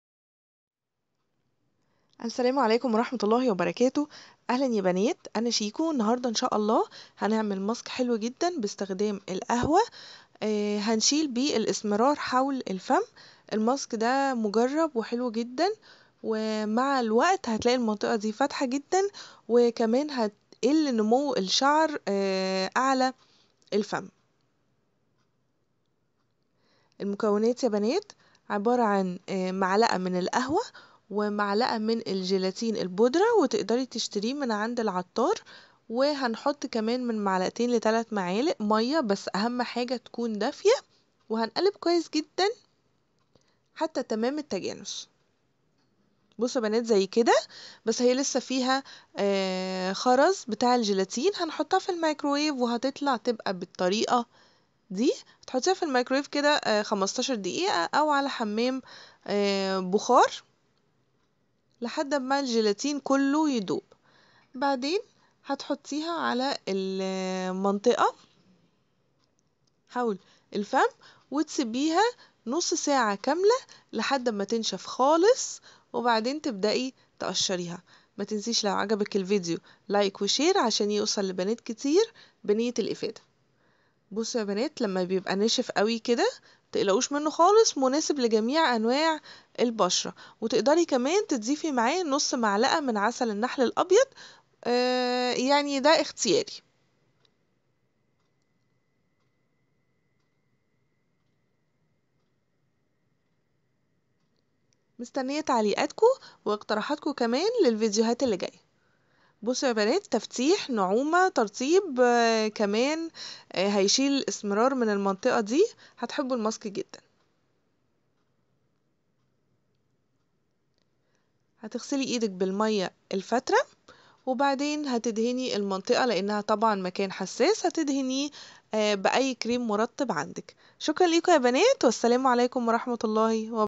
انزعيه طبقة واحدة لازالة السواد حول منطقة الفم |تفتيح|ازالة شعر في ان واحد |مع شيكو
ماسك ازالة السواد جربيه مش هتقدري تستغني عنه ابداا
تابعينى على حسابى الشخصى فى فيس بوك من هنا